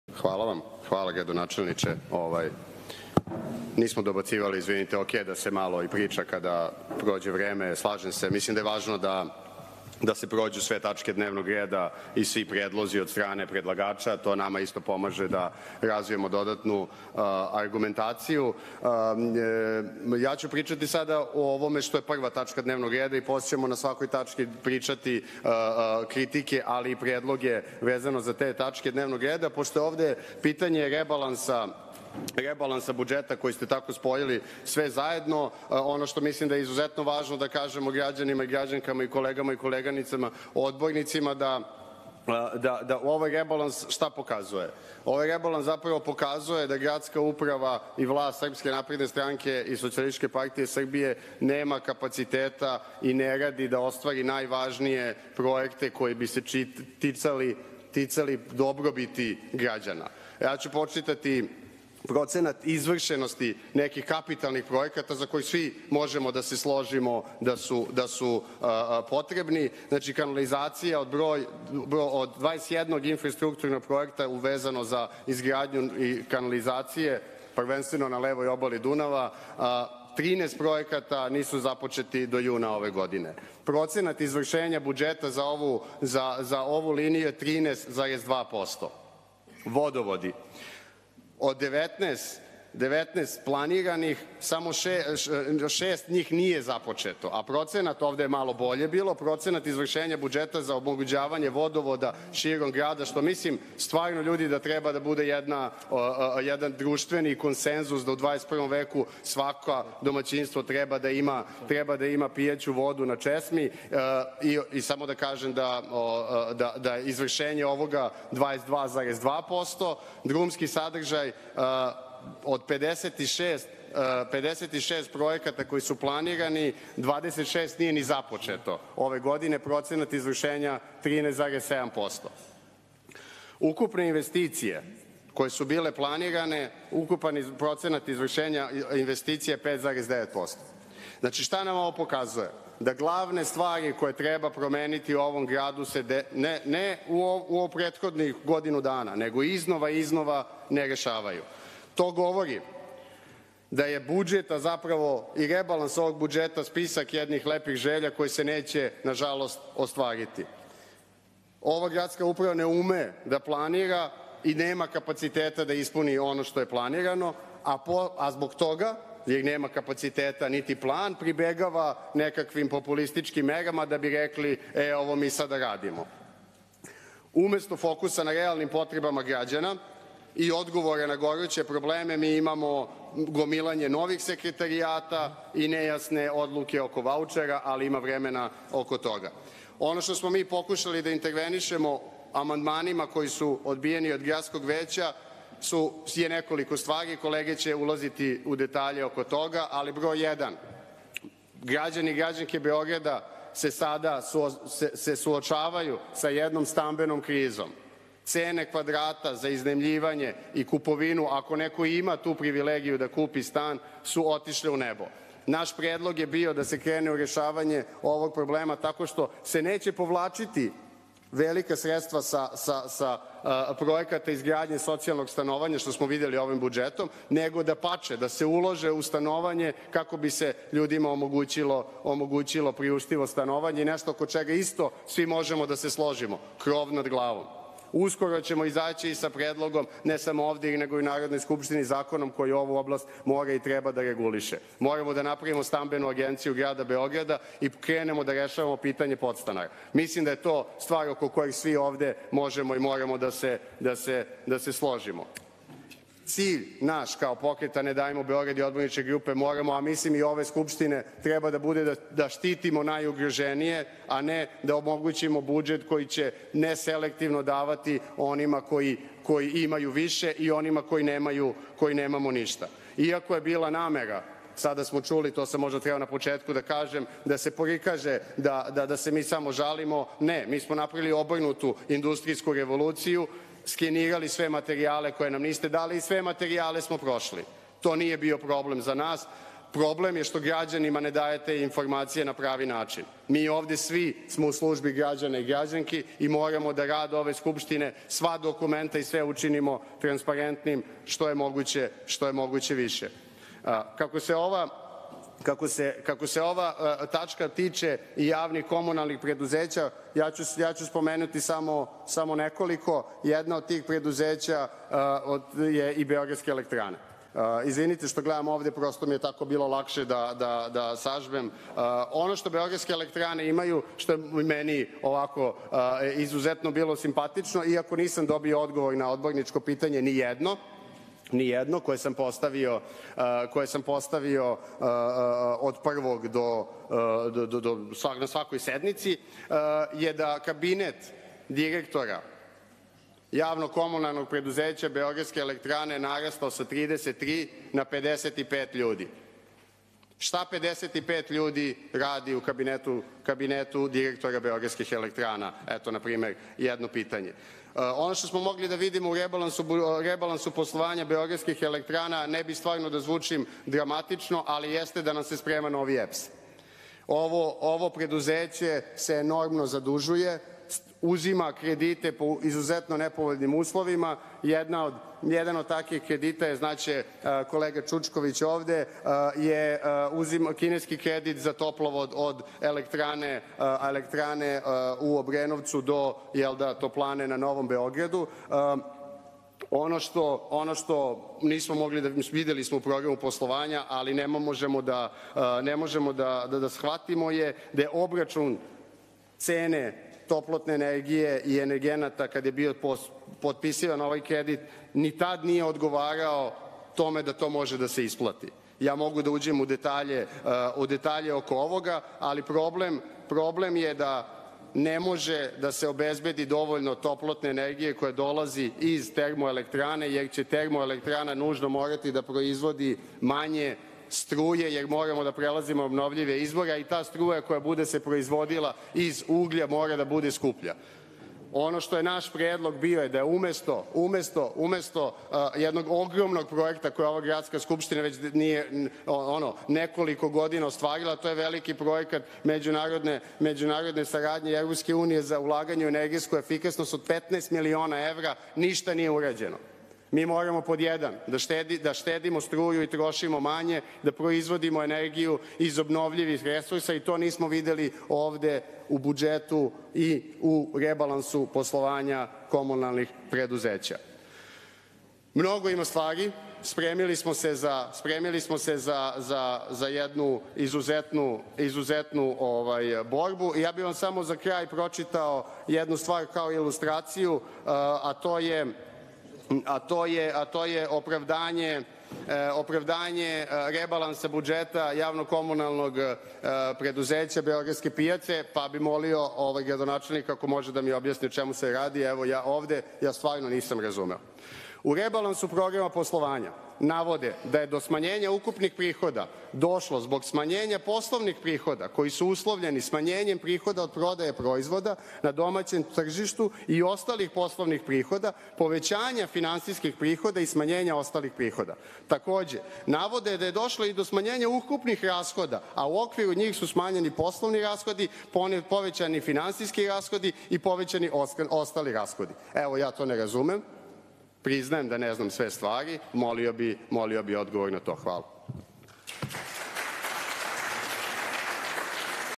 Veselinović: Cene nekretnina u Beogradu su otišle u nebo, postala je privilegija kupiti stan!
Poslušajte prvo obraćanje Dobrice Veselinovića na četvrtoj redovnoj sednici Skupštine grada Beograda.
Video isečak je vlasništvo televizije Studio B.
Učlanite se u Ne davimo Beograd, izborićemo se zajedno da Beograd postane dobar grad za sve
✅💚 Zaprati nas na mrežama: